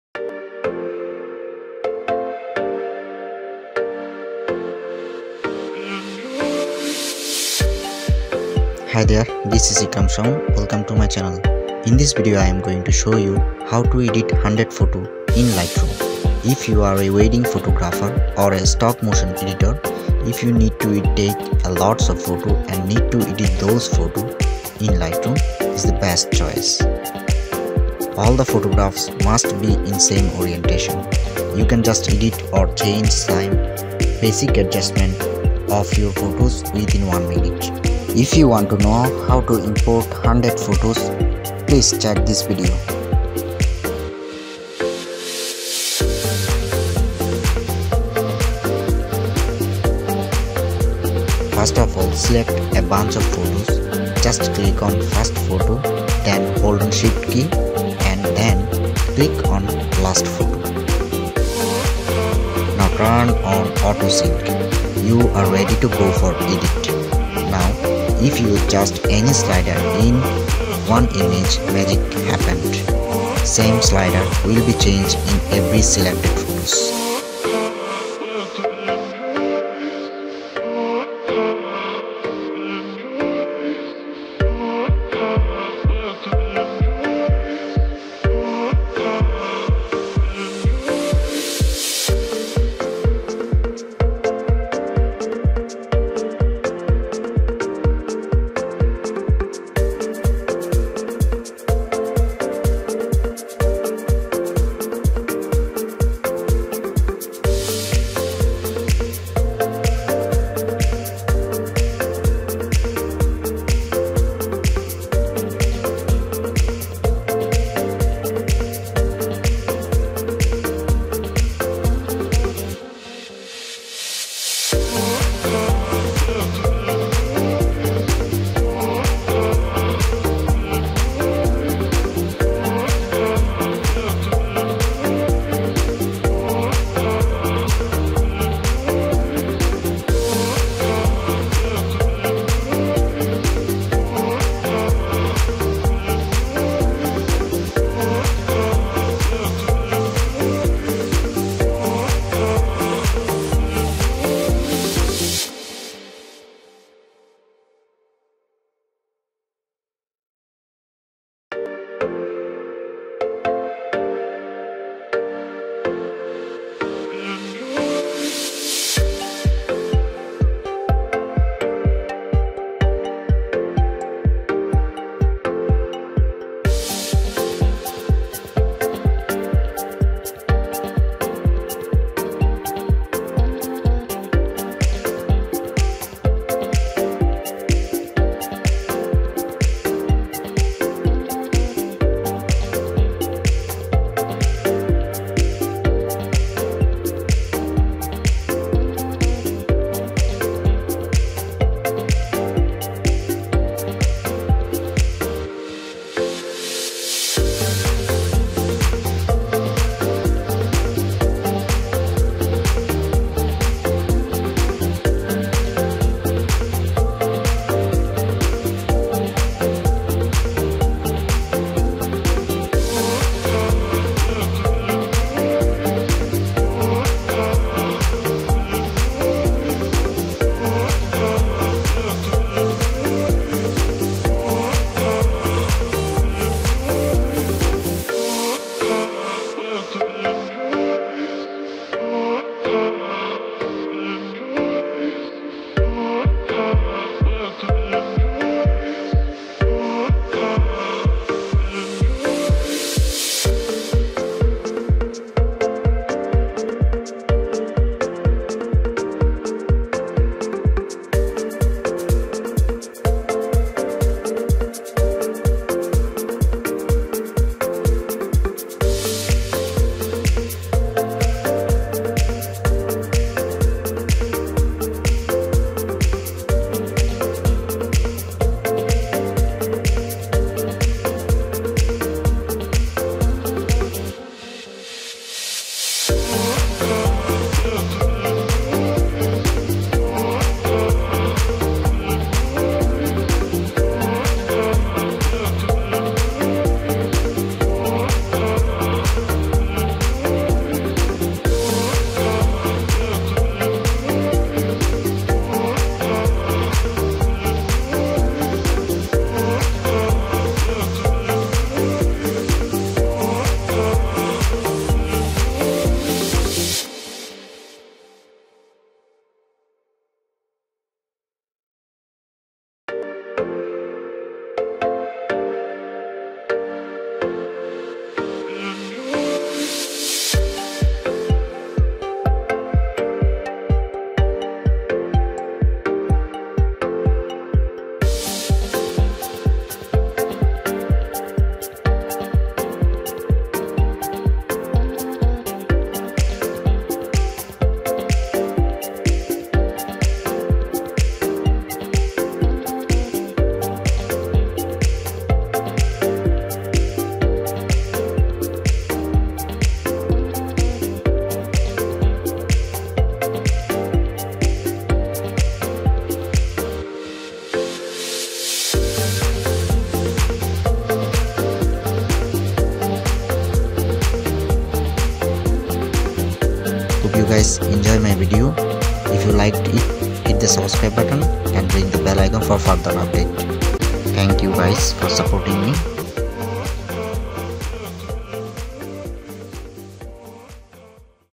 How to Edit 100 photo in 1 click | Lightroom pc tutorial for beginners
Hello everyone.
This is      @iqram shawon
Today I am going to show you How to edit 100 photos in just 1 click in lightroom.

Now let's get started with  adobe lightroom.